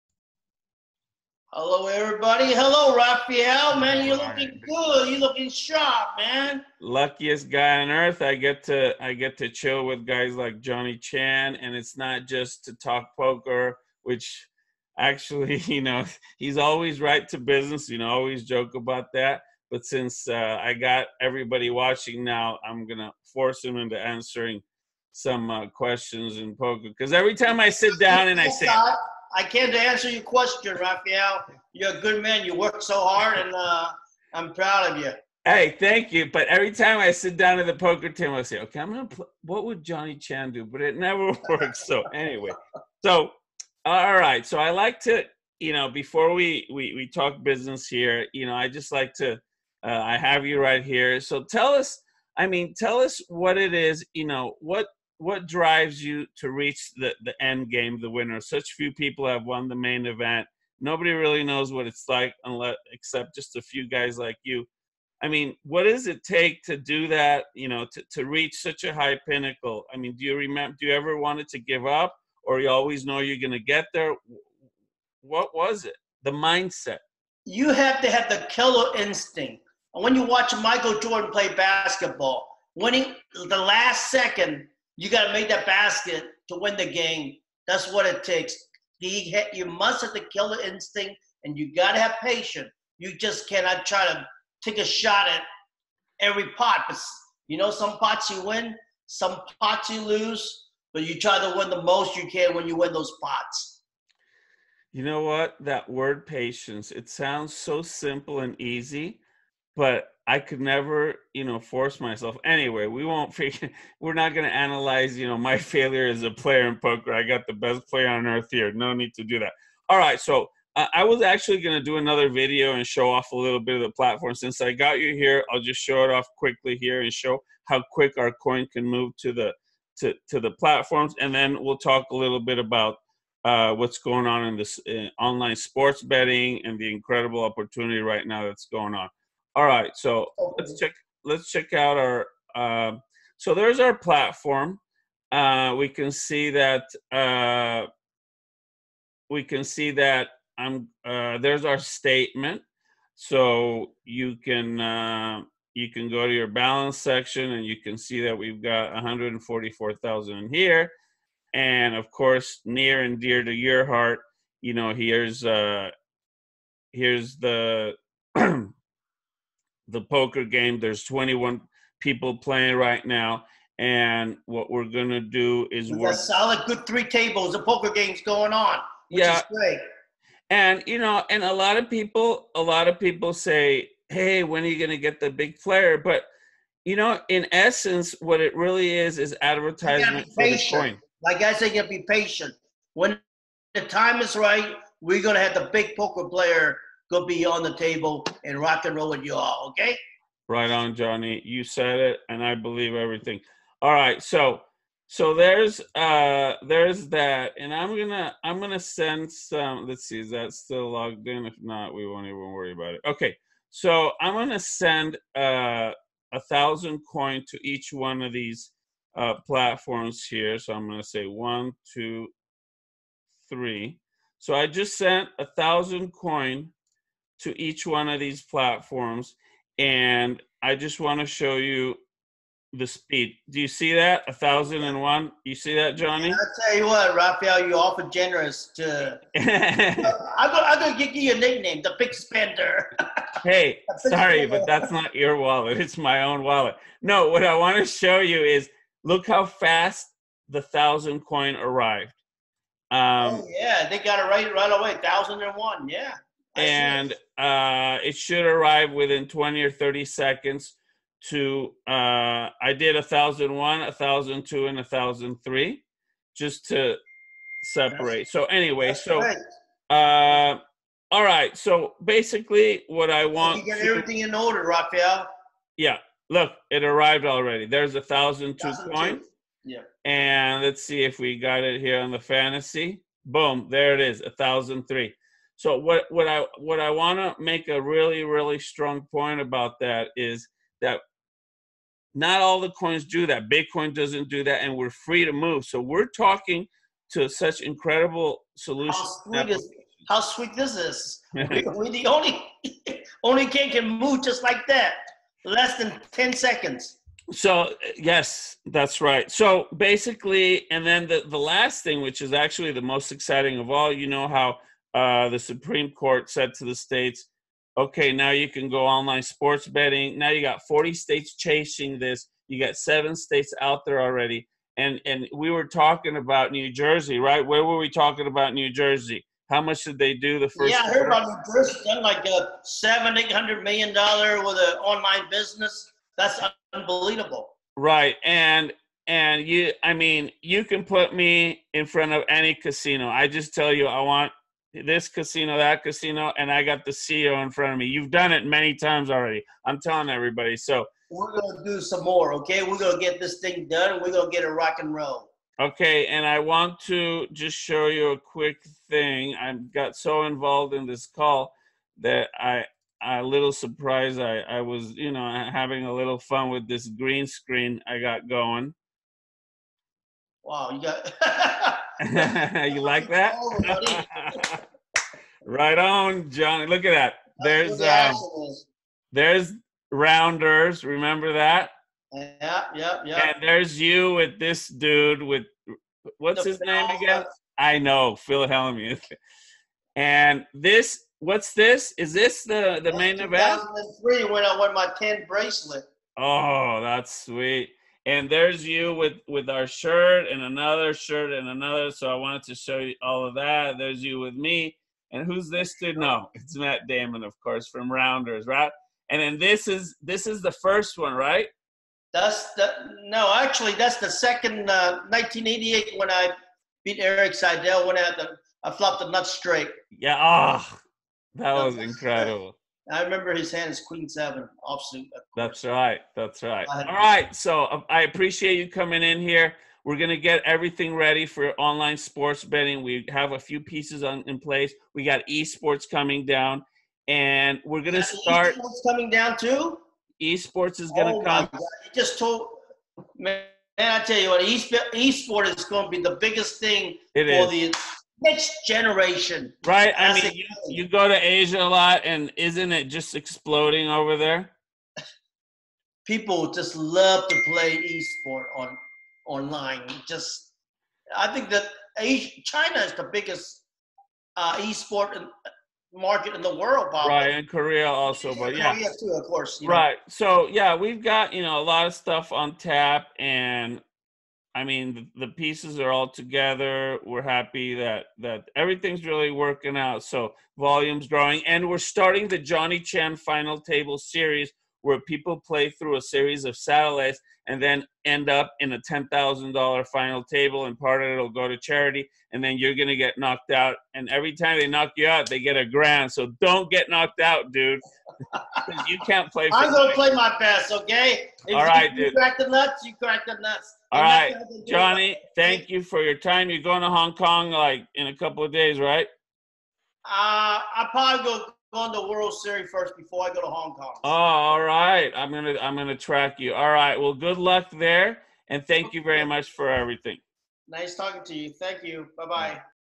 NLC2 Dev talks business and poker with the legend Johnny Chan
Rafael Groswirt, talks to Johnny about NLC2 and shows off how fast the coin is to reach the platforms. For more about NLC2 visit  Nolimitcoin.org  . NoLimitCoin powers nolimitfantasysports.com and nolimitcoinpoker.com .Johnny goes into memory lane after Rafael puts up a wall of iconic Johnny Chan pictures and Johnny describes them.